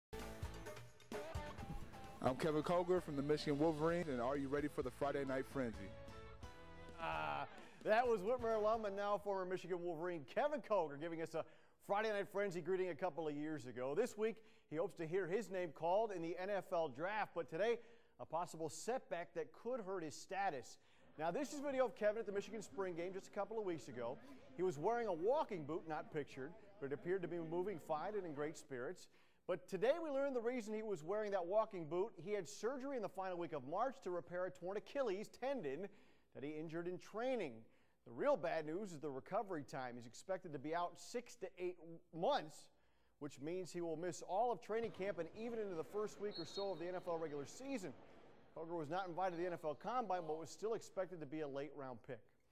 Torn achilles could impact Whitmer alums NFL draft status
Whitmer alum, Kevin Koger was in a walking boot at the Michigan spring game just over a week ago.

On Monday, we learned why.

Koger has a torn Achilles tendon and underwent surgery in the last week of March, after tearing it during pre-draft training.

The recovery time on a torn achilles could take anywhere from 6 months on average to 8 months to be in NFL shape.

The former University of Michigan tight end did not receive an invitation to the NFL Combine but was projected by some prognosticators to be a late round pick in this year's NFL draft.